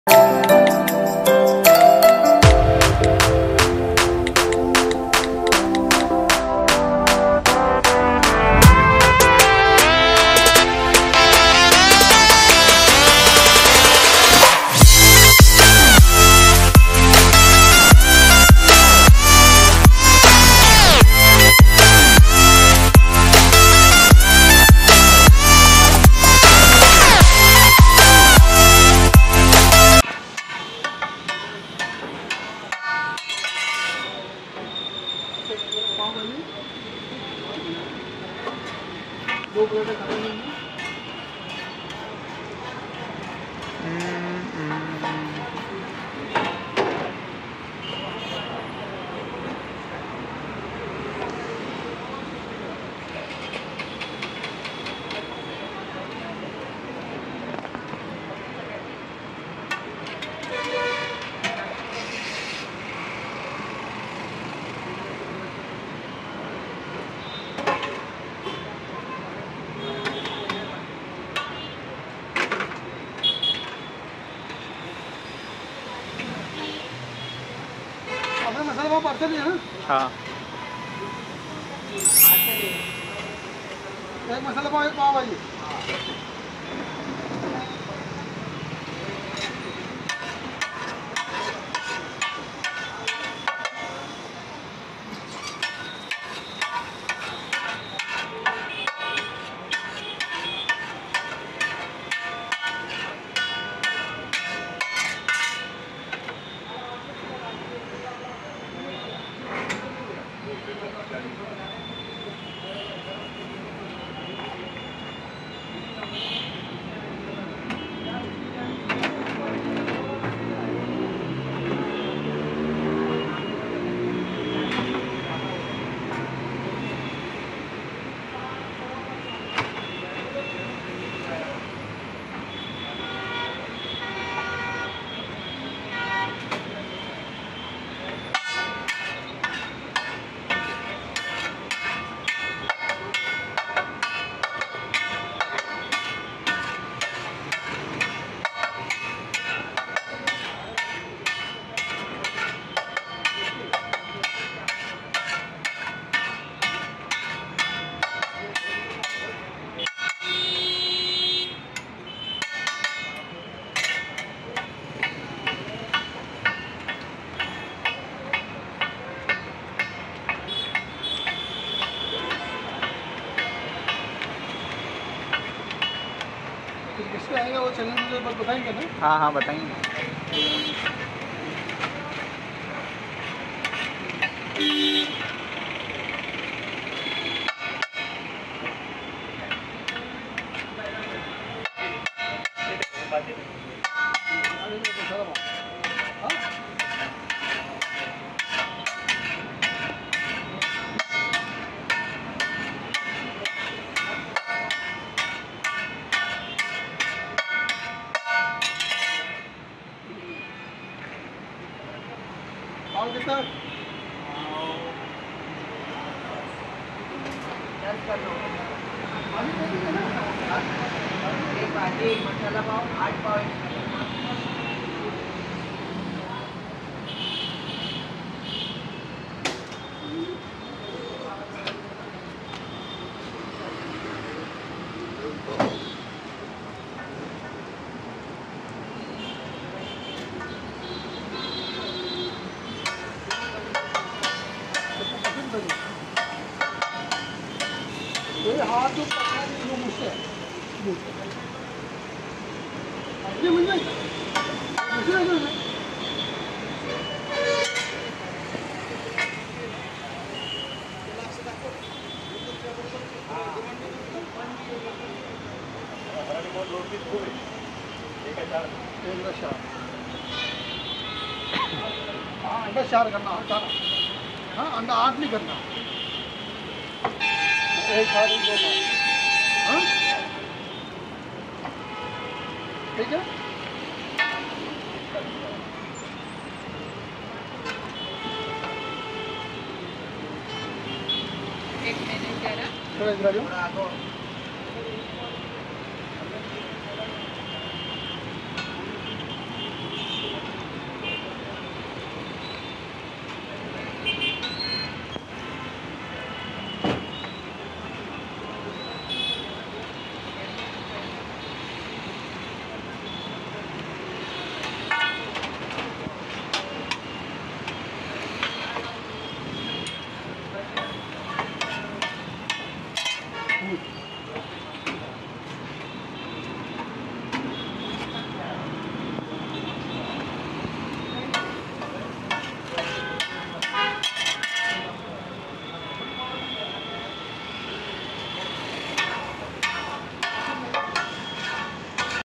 Spicy Mumbai Style Masala Pav Made with Amul Butter
Delicious Masala pav in Aurangabad Maharashtra

Place: Kailash Pav bhaji center, N3 CIDCO near cotton king store aurangabad Maharashtra 431001

Price : 90 ₹ /-

This place is one of the best place in Aurangabad to have
Pav bhaji
Tawa pulao
And famous Masala Pav (Mumbai style)
I'm coming here since my Childhood daysand it's the best for serving delicious food.